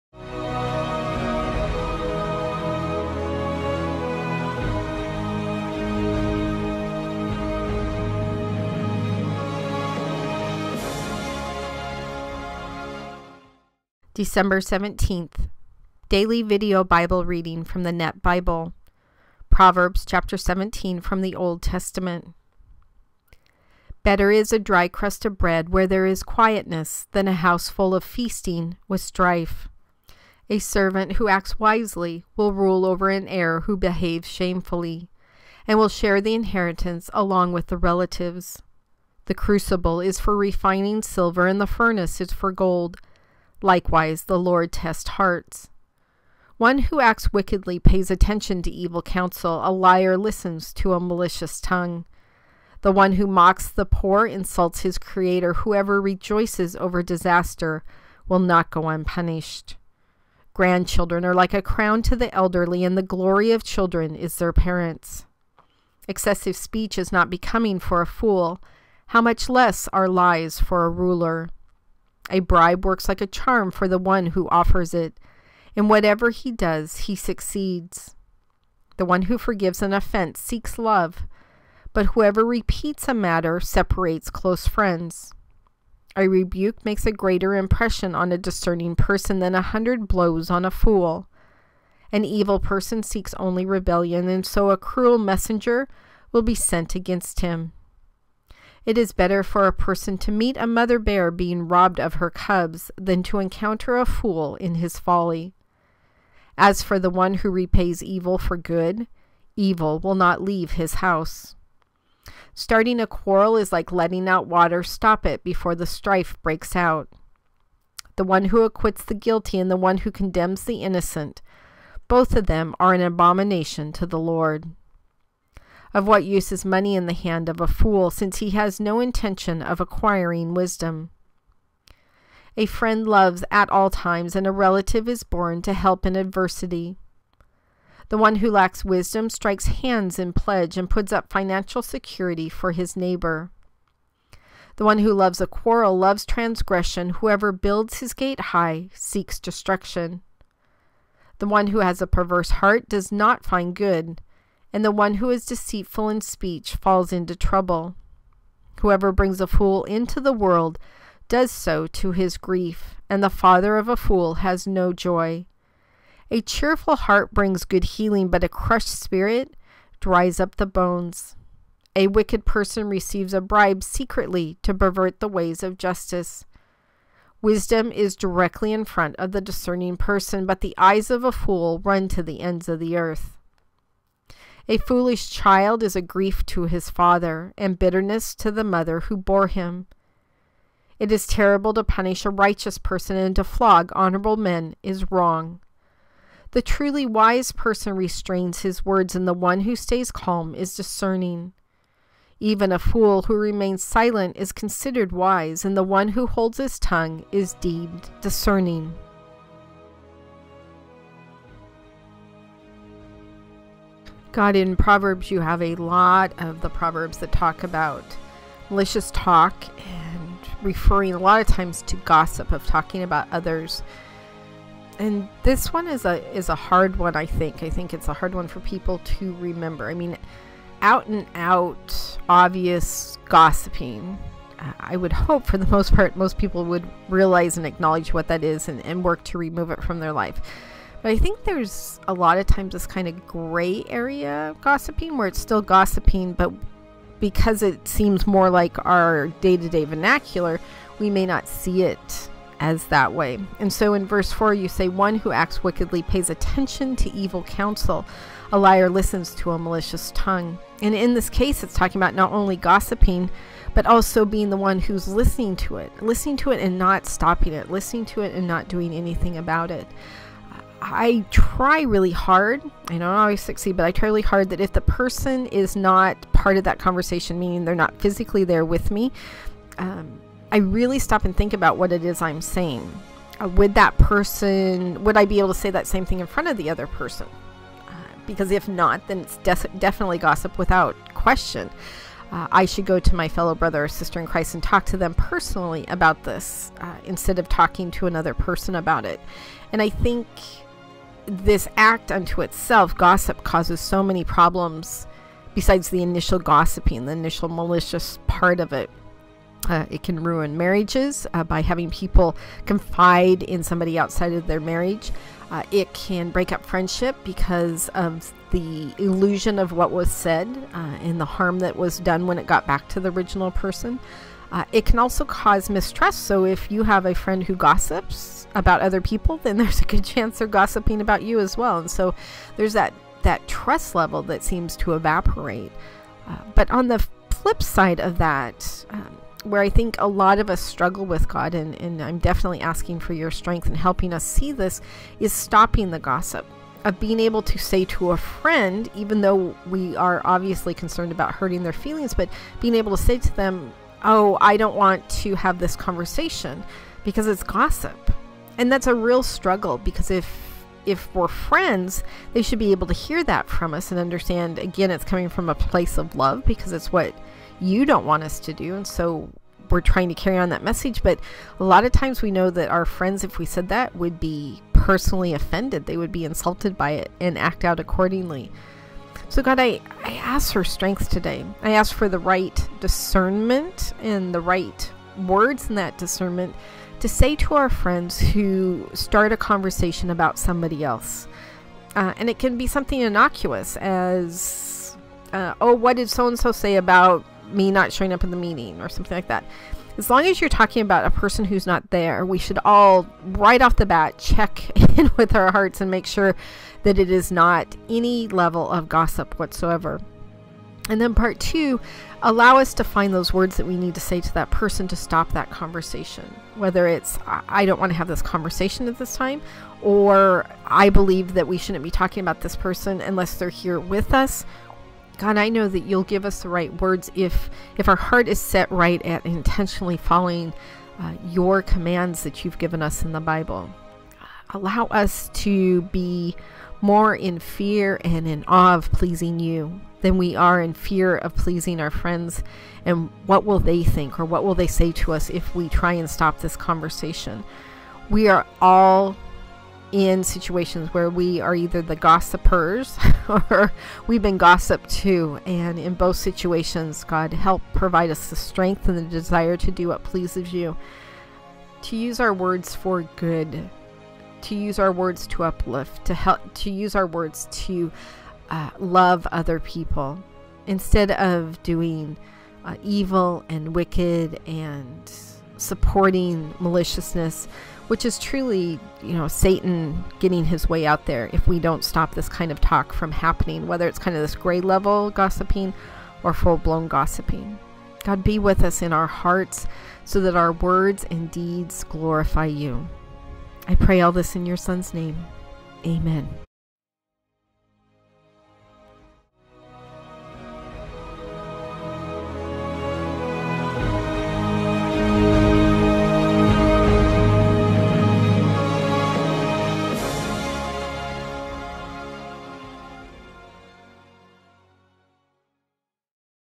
December 17 Proverbs 17 Old Testament for the Daily Video Bible DVB
Proverbs 17:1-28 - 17:1 Better is a dry crust of bread where there is quietness
than a house full of feasting with strife.
17:2 A servant who acts wisely will rule
over an heir who behaves shamefully,
and will share the inheritance along with the relatives.
17:3 The crucible is for refining silver and the furnace is for gold,
likewise the LORD tests hearts.
17:4 One who acts wickedly pays attention to evil counsel;
a liar listens to a malicious tongue.
17:5 The one who mocks the poor insults his Creator;
whoever rejoices over disaster will not go unpunished.
17:6 Grandchildren are like a crown to the elderly,
and the glory of children is their parents.
17:7 Excessive speech is not becoming for a fool;
how much less are lies for a ruler!
17:8 A bribe works like a charm for the one who offers it;
in whatever he does he succeeds.
17:9 The one who forgives an offense seeks love,
but whoever repeats a matter separates close friends.
17:10 A rebuke makes a greater impression on a discerning person
than a hundred blows on a fool.
17:11 An evil person seeks only rebellion,
and so a cruel messenger will be sent against him.
17:12 It is better for a person to meet a mother bear being robbed of her cubs,
than to encounter a fool in his folly.
17:13 As for the one who repays evil for good,
evil will not leave his house.
17:14 Starting a quarrel is like letting out water;
stop it before strife breaks out!
17:15 The one who acquits the guilty and the one who condemns the innocent --
both of them are an abomination to the LORD.
17:16 Of what use is money in the hand of a fool,
since he has no intention of acquiring wisdom?
17:17 A friend loves at all times,
and a relative is born to help in adversity.
17:18 The one who lacks wisdom strikes hands in pledge,
and puts up financial security for his neighbor.
17:19 The one who loves a quarrel loves transgression;
whoever builds his gate high seeks destruction.
17:20 The one who has a perverse heart does not find good,
and the one who is deceitful in speech falls into trouble.
17:21 Whoever brings a fool into the world does so to his grief,
and the father of a fool has no joy.
17:22 A cheerful heart brings good healing,
but a crushed spirit dries up the bones.
17:23 A wicked person receives a bribe secretly
to pervert the ways of justice.
17:24 Wisdom is directly in front of the discerning person,
but the eyes of a fool run to the ends of the earth.
17:25 A foolish child is a grief to his father,
and bitterness to the mother who bore him.
17:26 It is terrible to punish a righteous person,
and to flog honorable men is wrong.
17:27 The truly wise person restrains his words,
and the one who stays calm is discerning.
17:28 Even a fool who remains silent is considered wise,
and the one who holds his tongue is deemed discerning.